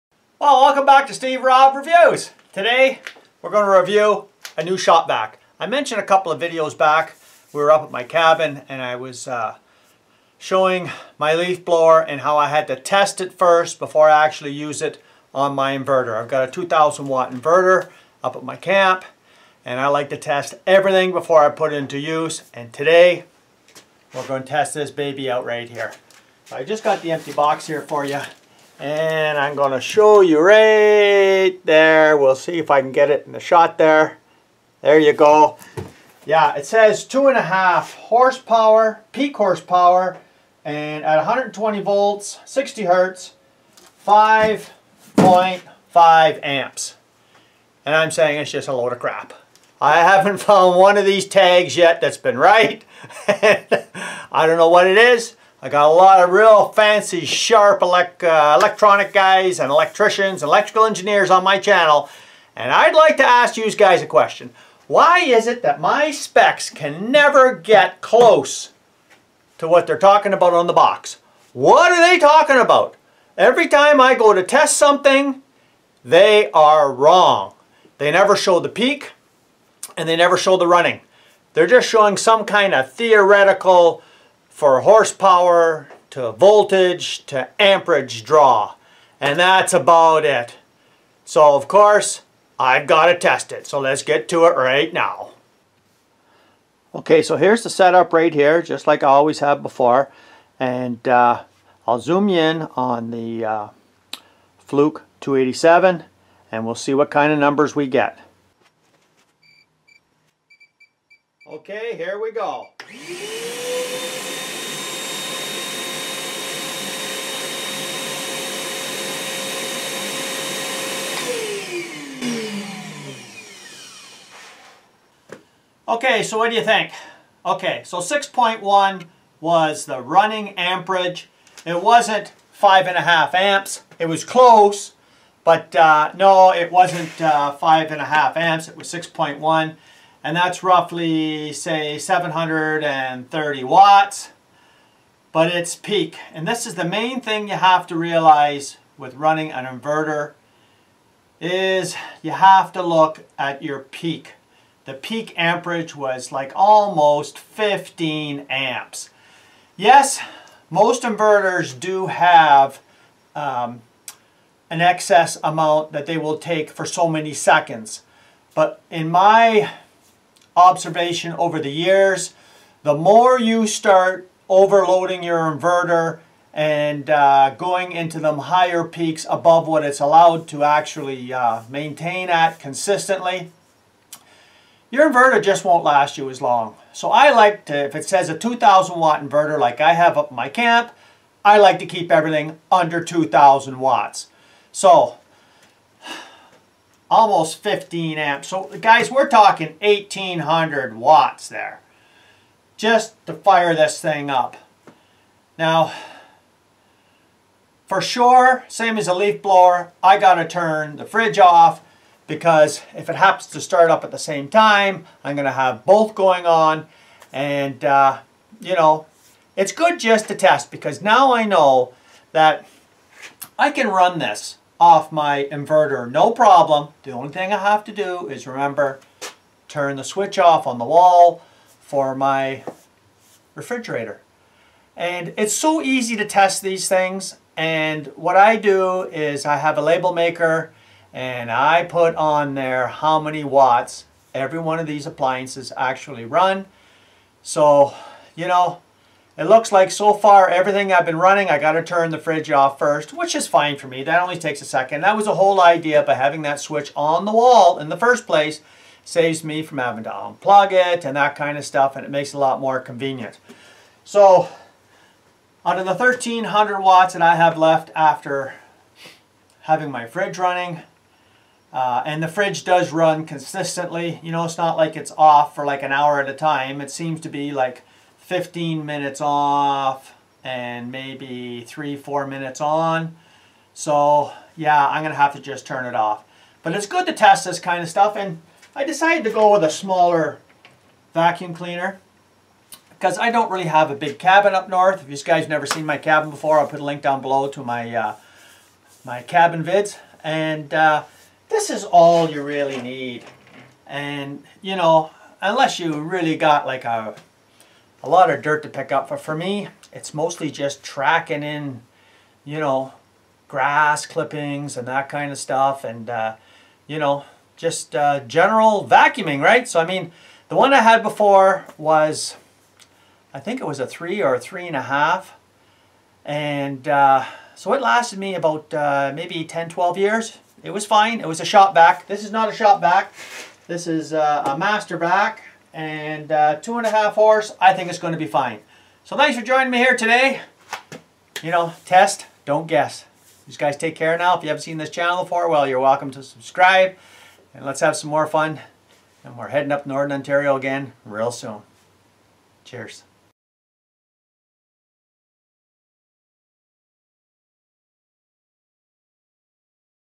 How To Test Amperage Draw On A Shop Vac
THESE ARE MY VIDEOS AT MY OFF GRID CABIN - LINK ABOVE

Today is testing for maximum and running amperage on a shop vac for my off grid  cabin in Northern Ontario. I have a 2000 watt power inverter . In my experience you must test and not guess if an electrical appliance is within your wattage requirement of your power inverter .  The fluke 287 and the fluke amp clamp do a terrific job at capturing the amperage draw results .

If you'd like to see what's happening in upcoming videos check out my instagram page.